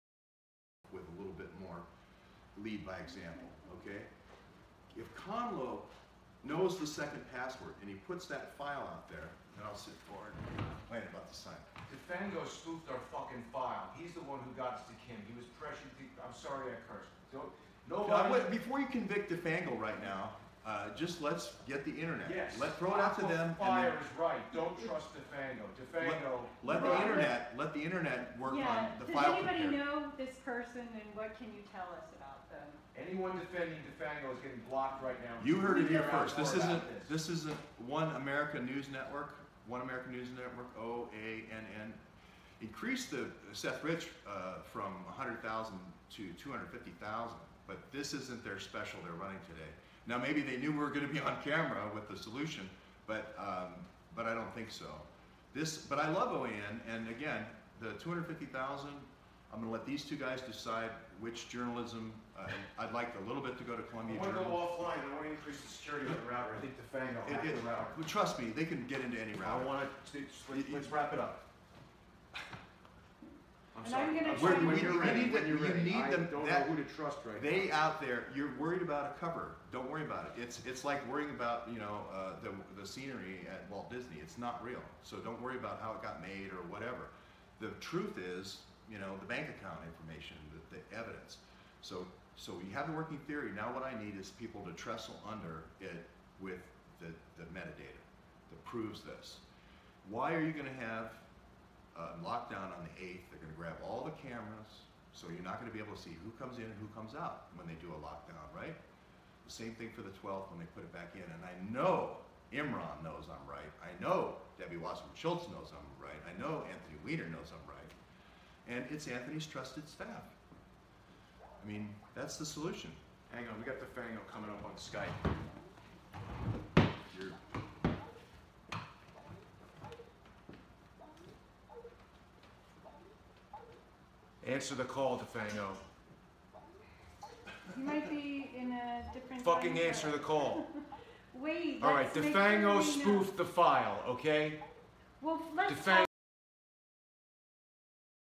Jason Goodman calls Defango a spoofer
This morning Jason Goodman went off on Defango, claims of altering and spoofing files, even claims of hacking his router. Here is the footage.

Something is up, but is this just a distraction to distract focus from the external forces of who's pulling the strings? Time will tell.

Here's the source Last 2.5 minutes (may have come up a little earlier).